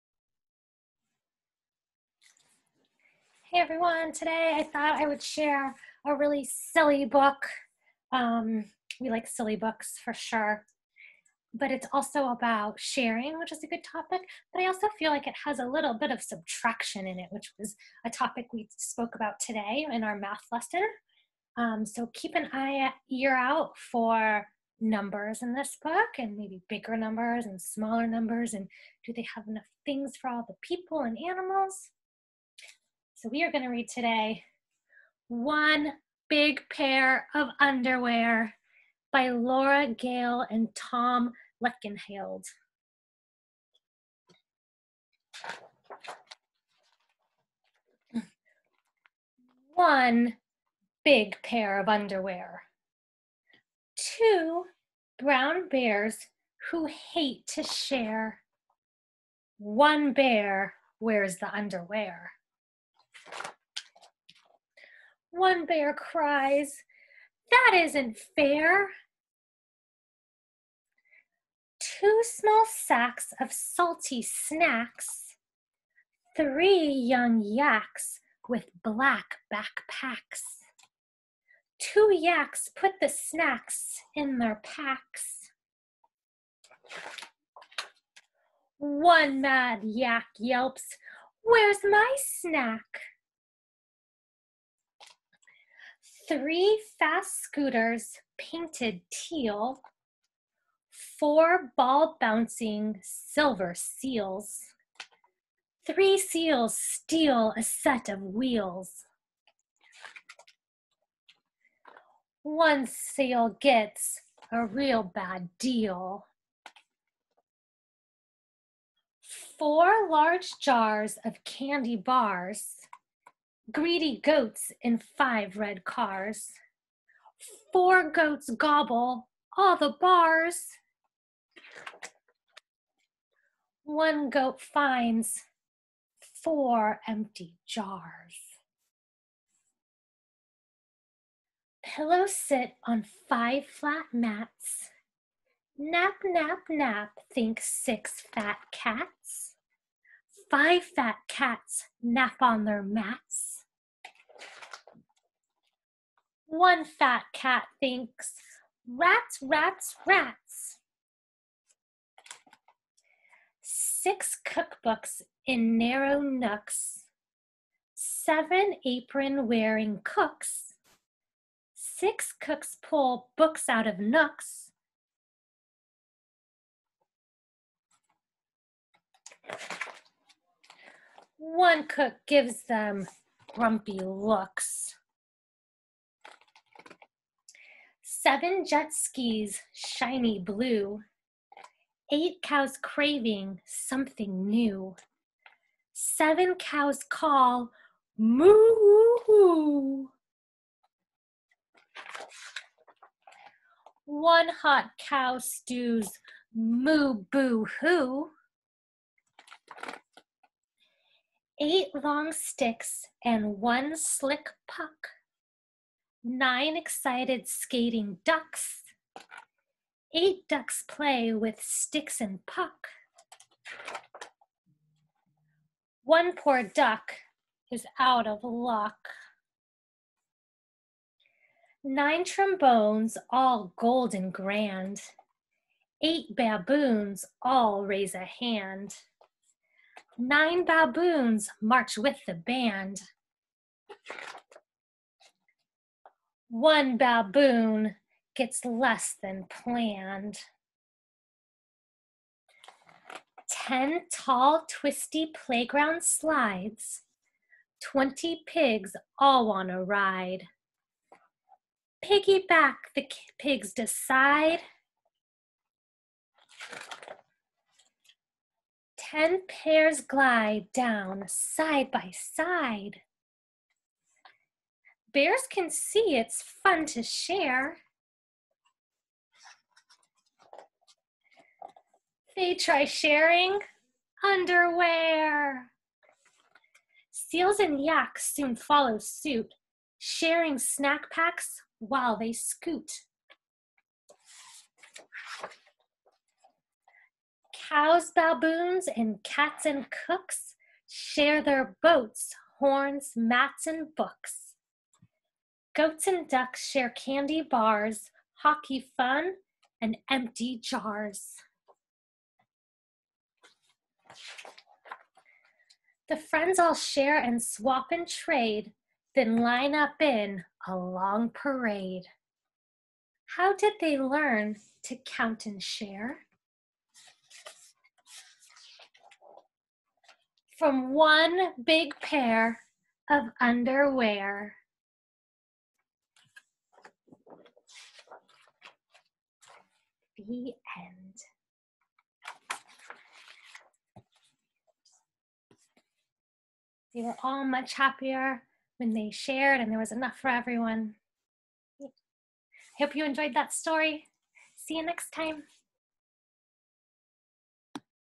A silly counting & sharing story
Join Teacher Jessica as she reads "One Big Pair of Underwear".